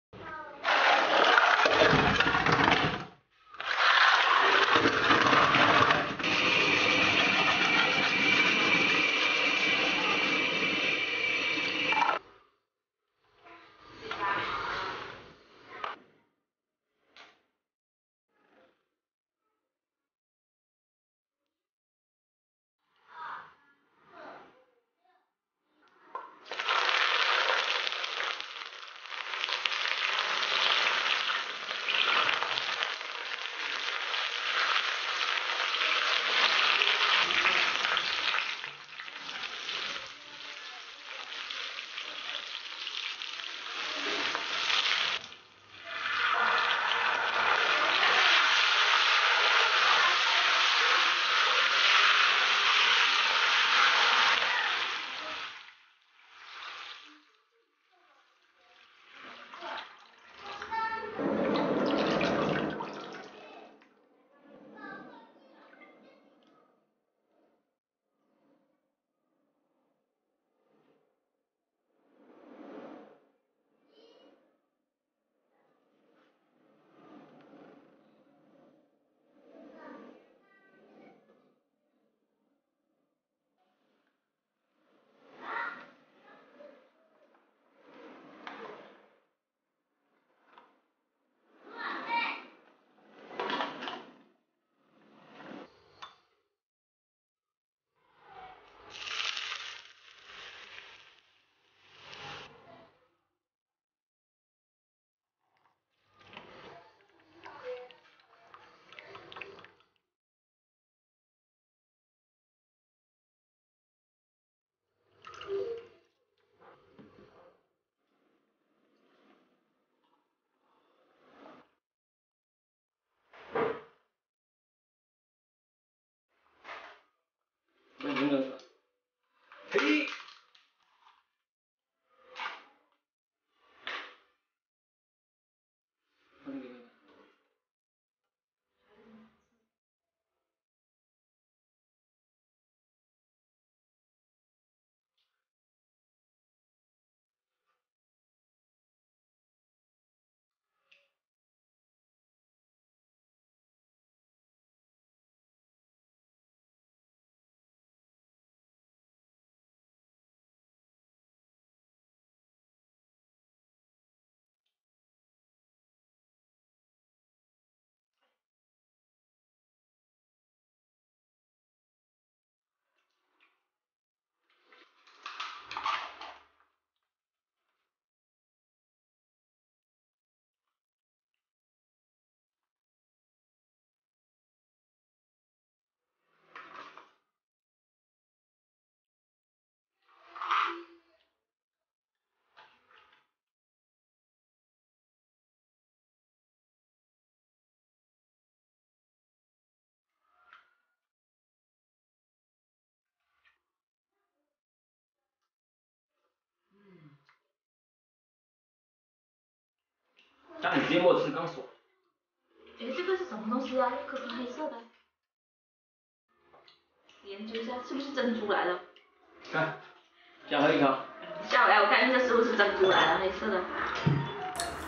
鲜贝泛滥的季节，买了一大锅才30多，味道鲜美，把老婆吃撑了
分享各种做法简单又好吃的美食做法